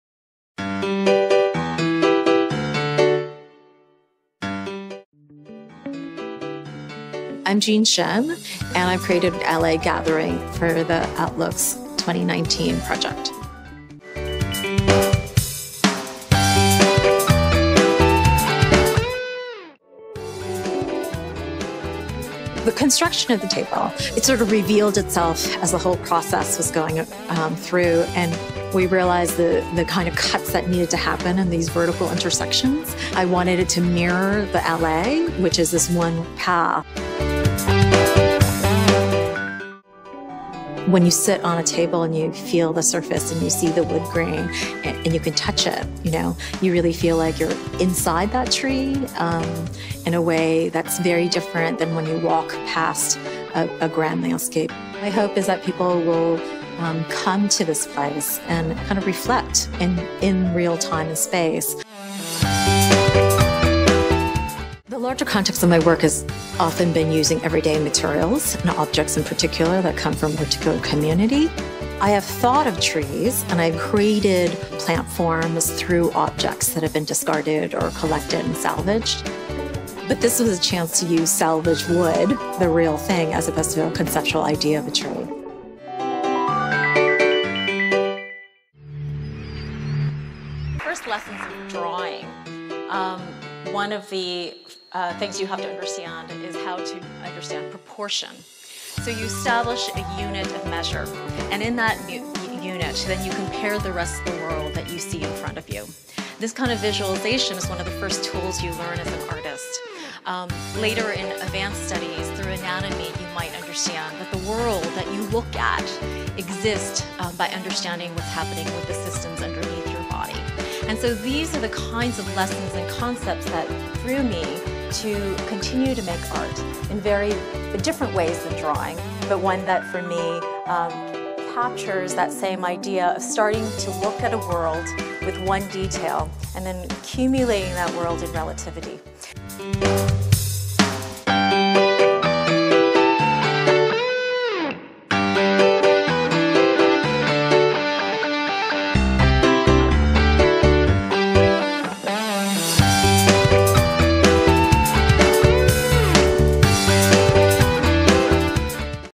Allee Gathering by Jean Shin - Storm King Art Center
To see more of the interactive and fun video of Allee Gathering by Jean Shin click this

Visit New York and see Allee Gathering by Jean Shin at Storm King Art Center

Allée Gathering, Shin’s project is a tribute to the Art Center’s beloved Maple Allée and will offer a new, communal place for visitors to come together in conversation to observe and reflect upon the changing landscape. The new black gum trees will be planted along the Allée, replacing the maple trees. The original maple tree stumps will remain in place for the duration of the exhibition, visually marking the landscape’s transition.

Jean Shin is an American artist living in Brooklyn, NY. She is known for creating elaborate sculptures and site-specific installations using accumulated cast-off materials.